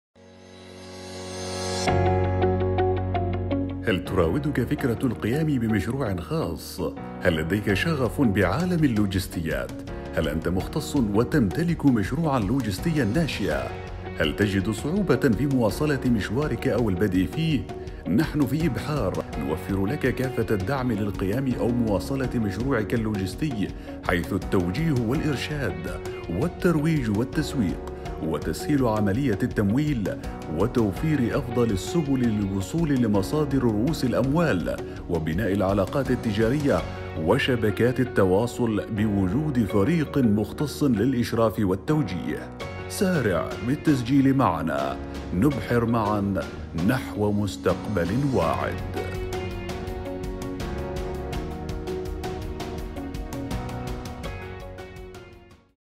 حـاضنة إبحــار
نحتضن في إبحار الأفكار الإبداعية في مجال اللوجيستيات لتصبح منتجات أو خدمات تجارية ناجحة

:سارع في التسجيل معنا، نبحر معاً مع نحو مستقبل واعد